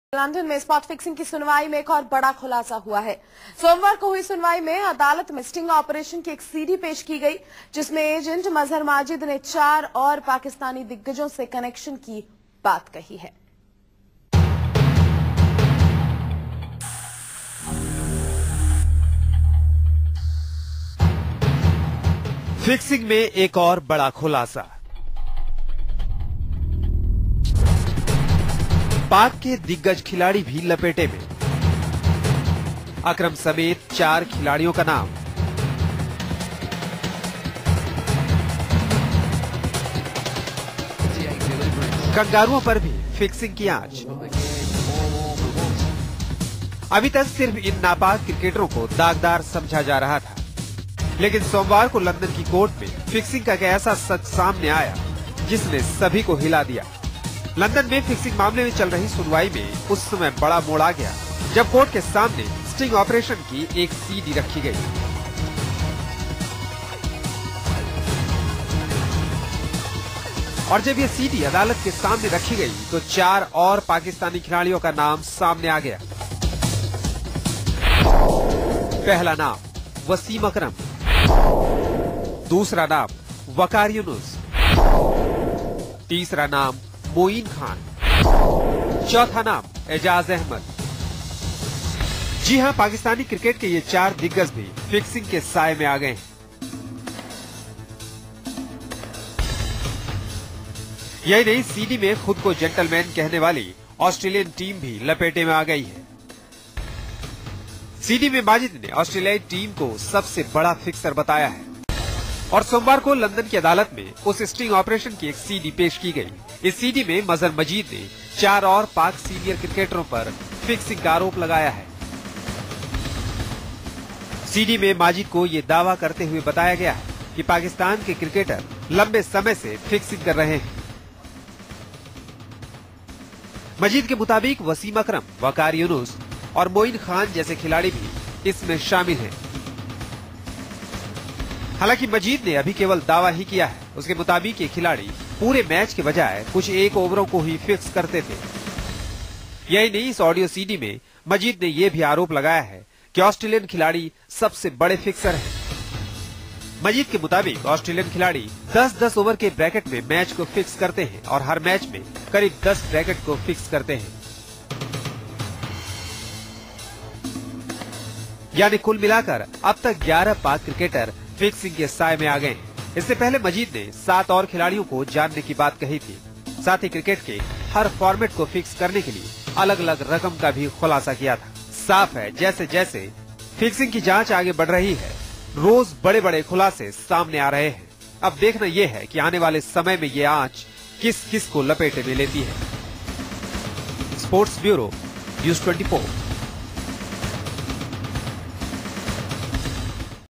Cricket agent boasts that SEVEN Pakistani players were fixing
A cricket agent boasted seven players in Pakistan's national side were fixing matches for him in a hugely lucrative betting scam, a court heard today.